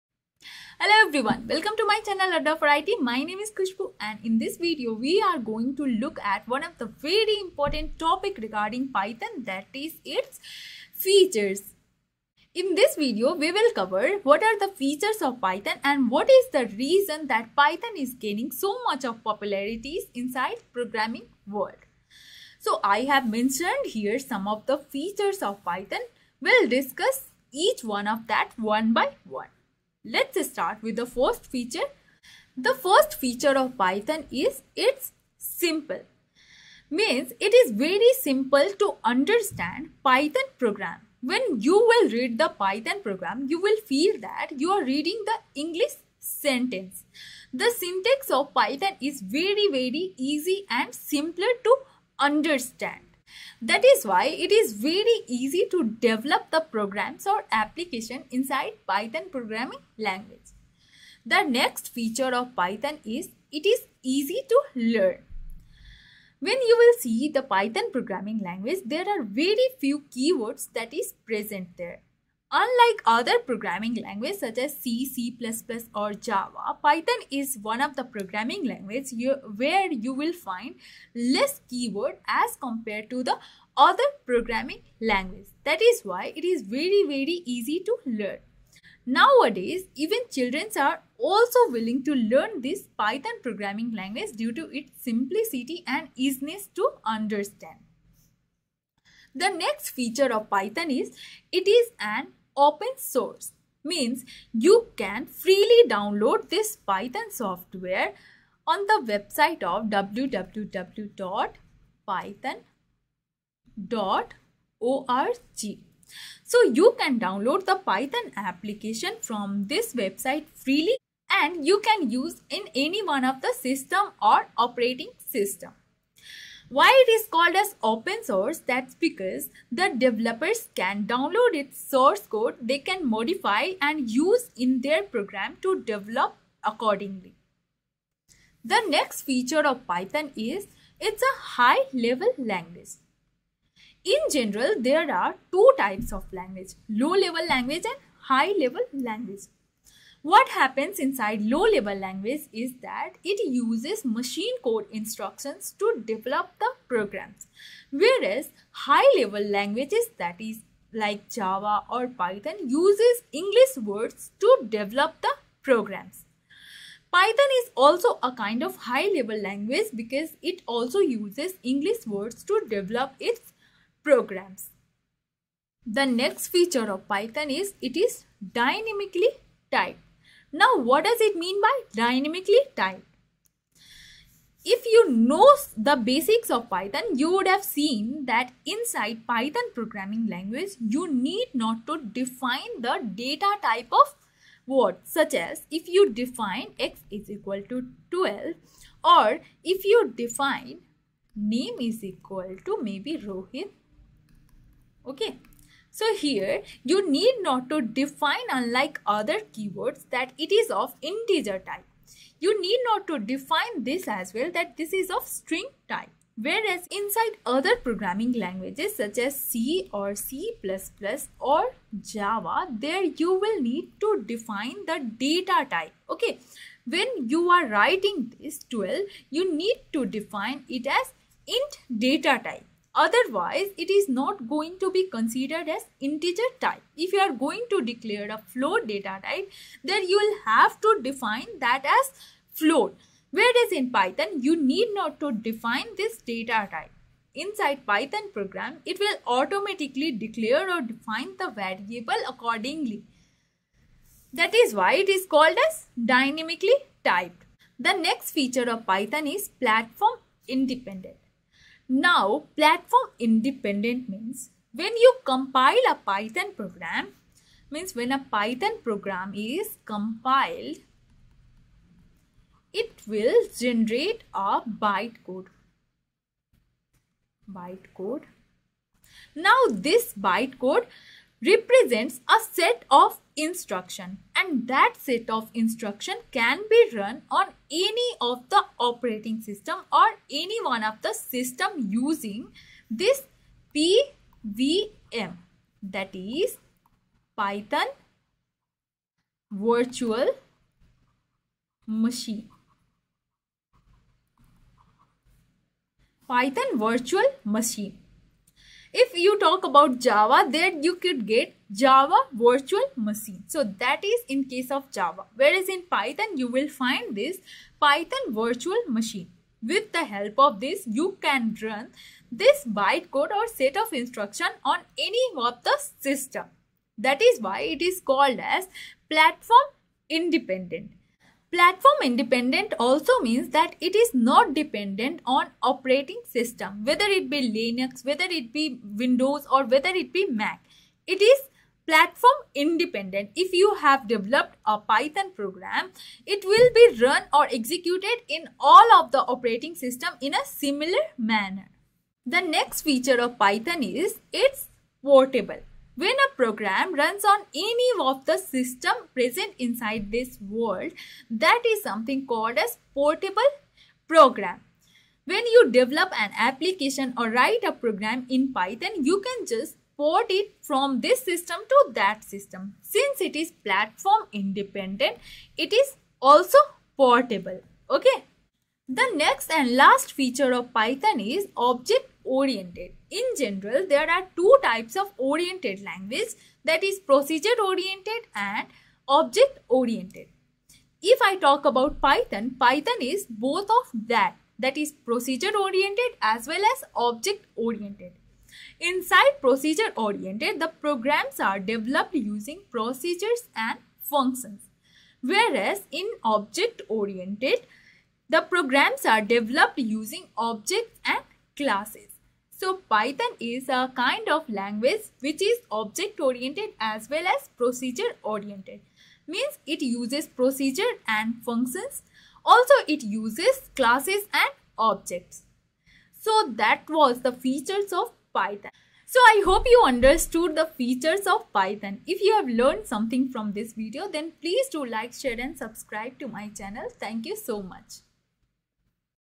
Features of Python || Simplest Explanation
This video explains the features of python  in very very detailed manner.
Python Features:-
Simple
Easy to learn
open source
High level language
Dynamically typed
Platform independent
Portable
Object oriented